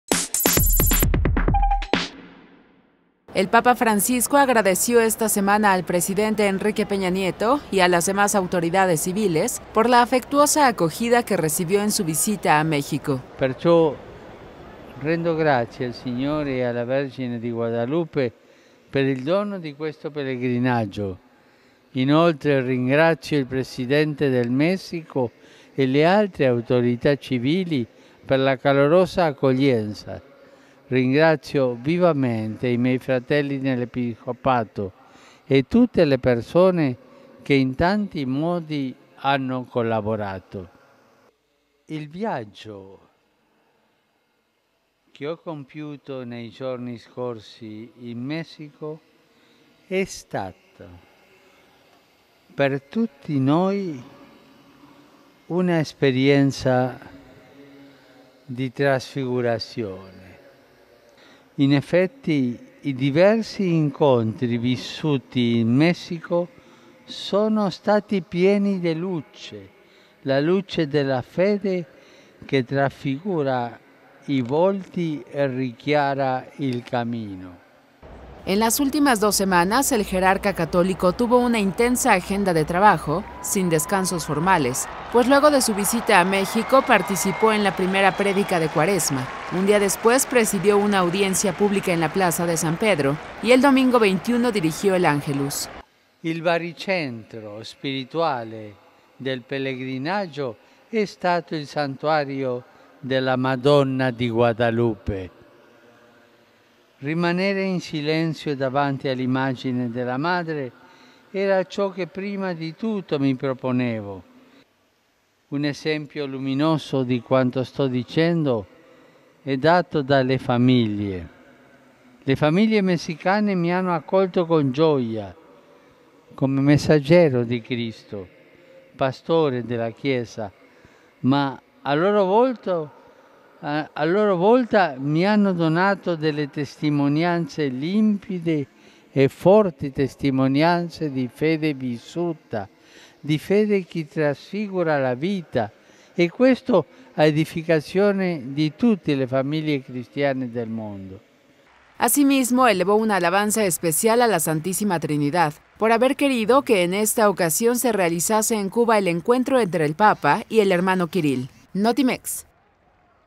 Papa Francisco agradece a Peña Nieto afectuoso recibimiento en México
Asimismo, elevó una alabanza especial a la Santísima Trinidad "por haber querido que, en esta ocasión, se realizase en Cuba el encuentro entre el Papa y el Patriarca de Moscú y de todas la Rusias